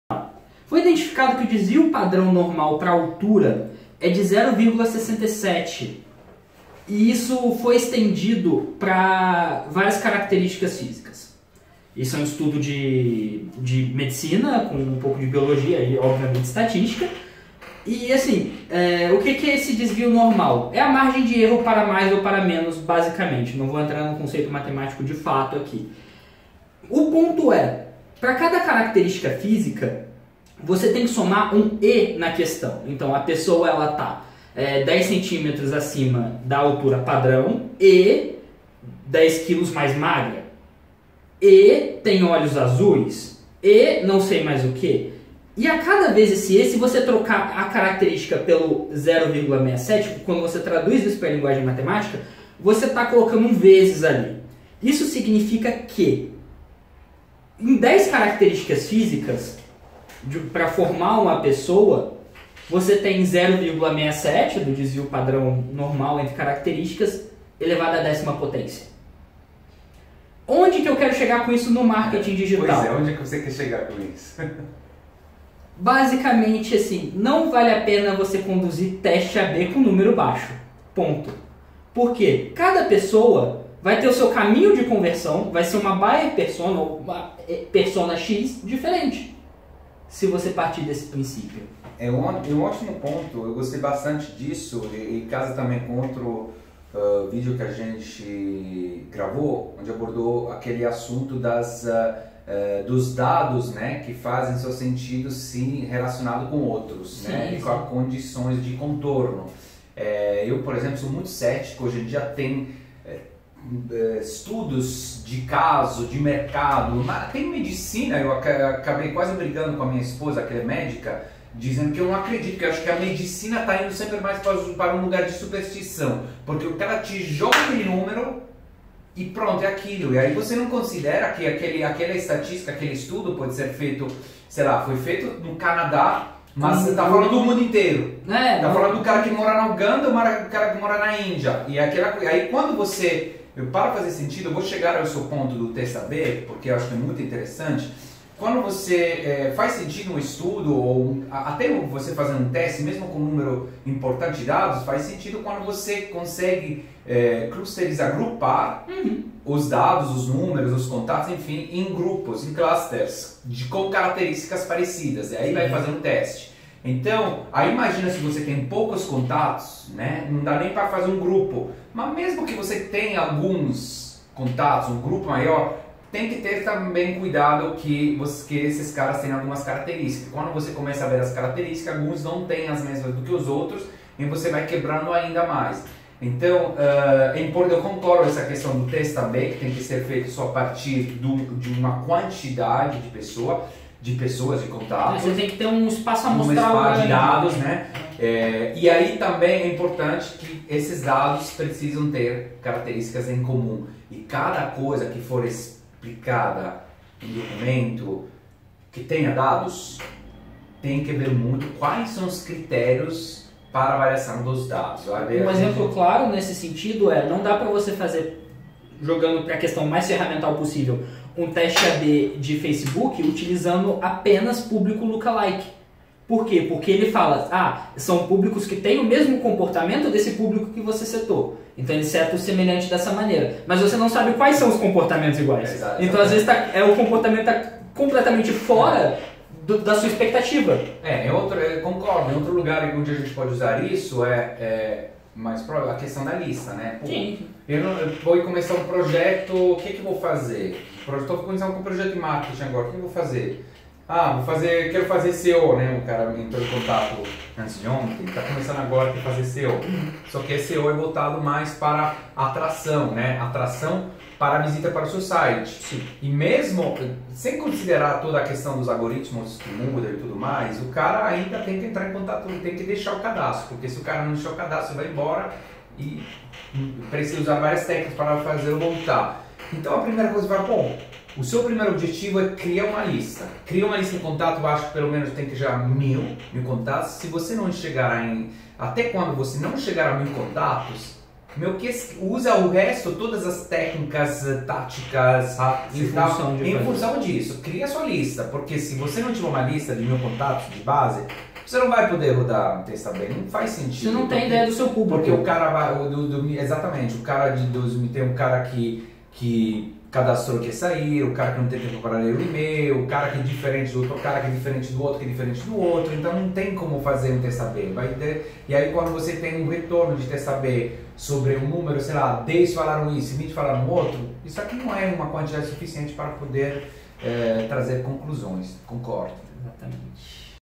Teste A/B para Marketing Digital - Café com Marketing
Teste A/B em Marketing: Sabia que até os estudos de biologia evolutiva podem nos ensinar sobre o assunto?
Muito, acredite!

Vem com a gente pois explicamos tudinho em mais um Café com Marketing.

Para saber mais sobre estratégias de marketing digital acesse o site da NTP